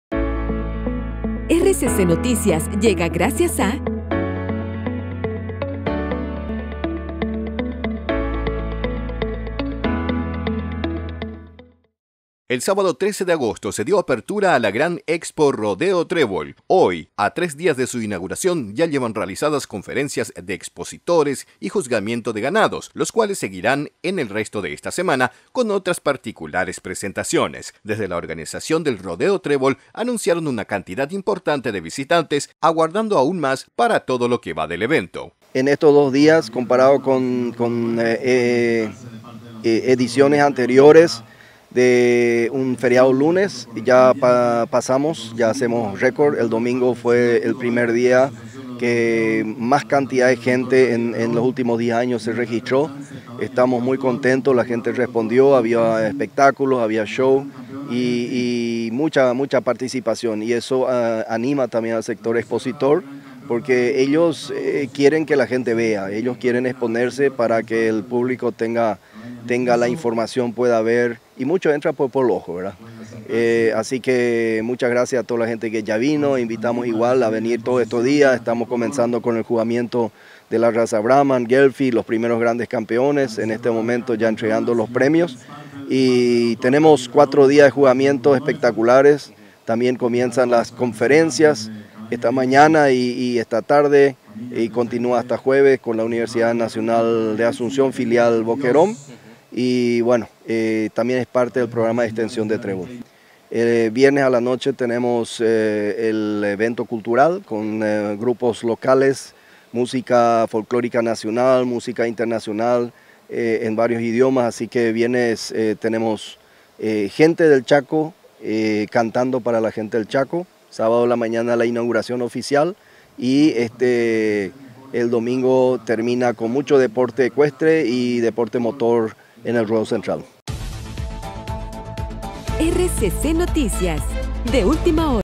Despliegue de la Expo Rodeo Trébol a tres días de su apertura
El sábado 13 de agosto, se dio apertura a la gran Expo Rodeo Trébol, hoy, a tres días de su inauguración ya llevan realizadas conferencias de expositores y juzgamiento de ganados, los cuales seguirán en el resto de esta semana, con otras particulares presentaciones. Desde la organización de Rodeo Trébol, anunciaron una cantidad importante de visitantes, aguardando aún más para todo lo que va del evento.